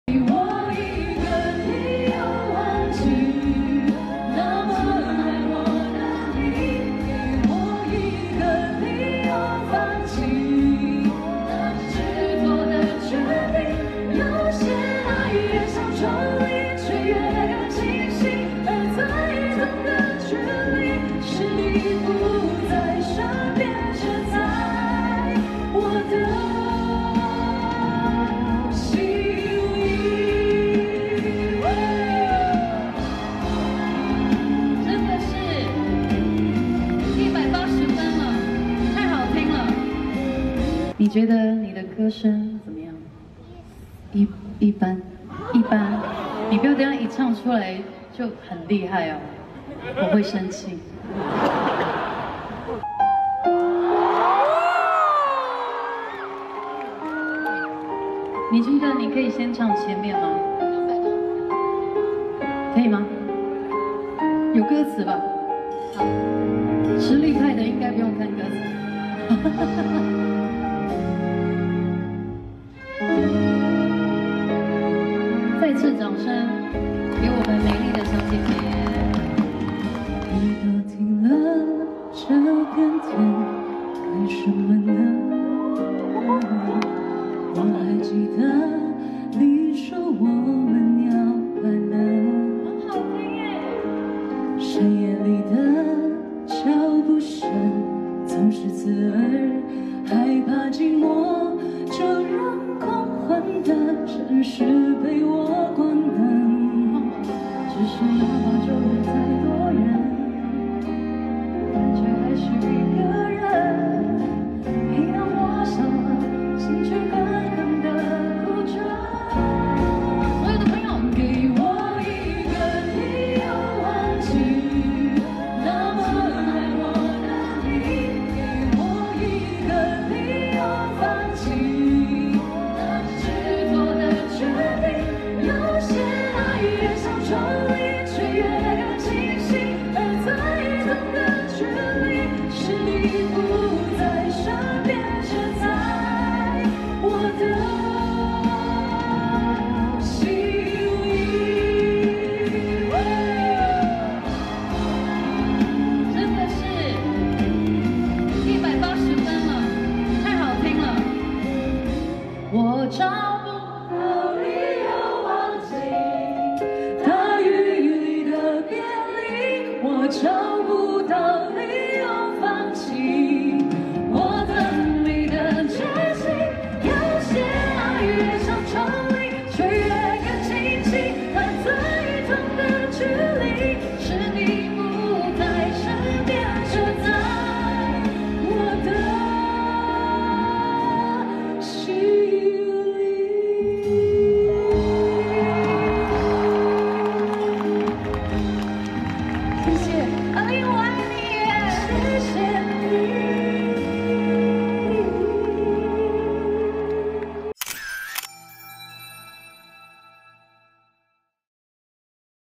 A-Lin演唱會遇粉絲合唱 一鳴驚人網聽跪求原地出道｜三立娛樂星聞
小紅書 / 親愛的Annye
A-Lin